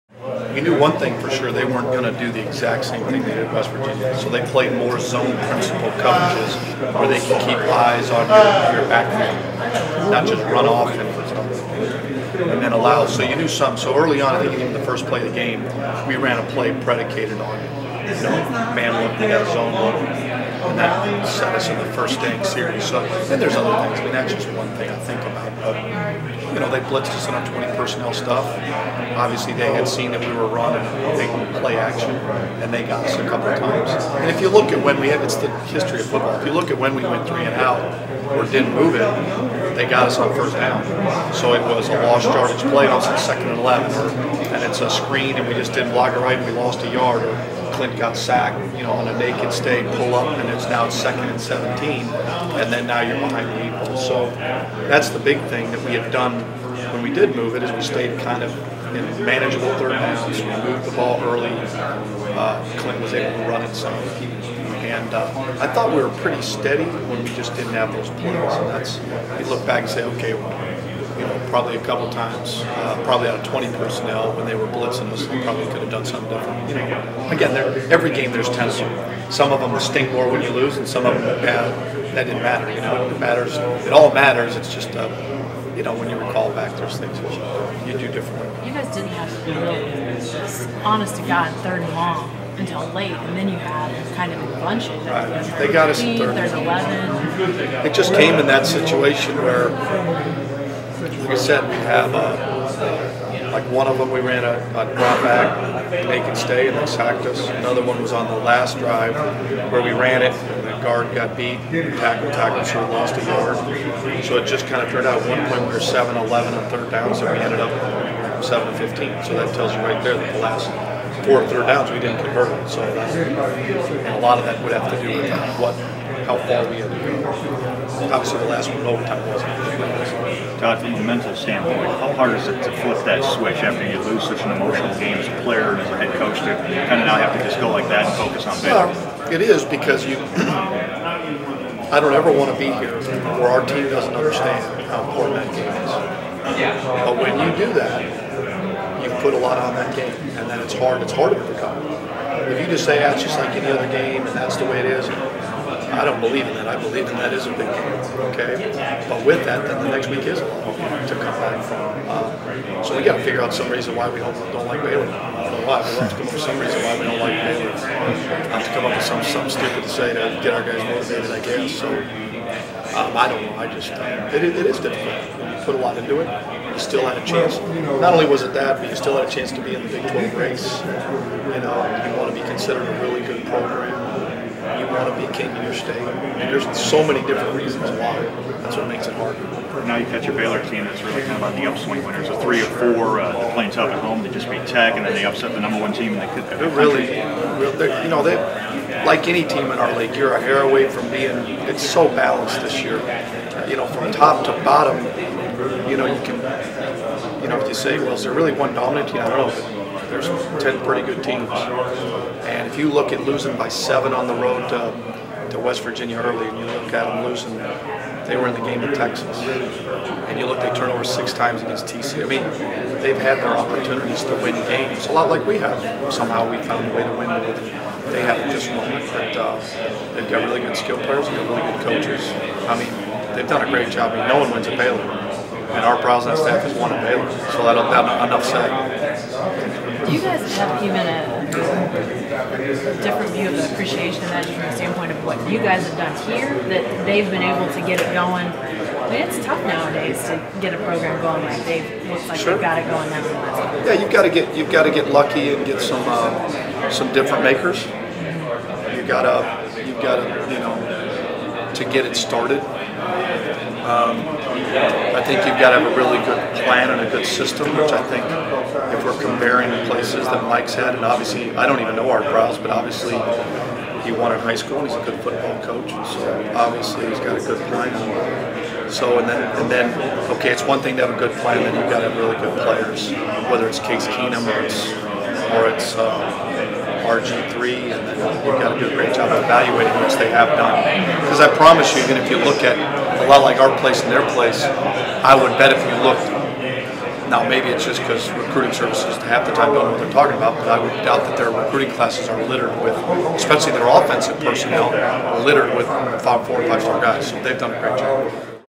Todd Monken Interview (November 26, 2012)
Oklahoma State offensive coordinator Todd Monken speaks to the press at the November 26 media luncheon.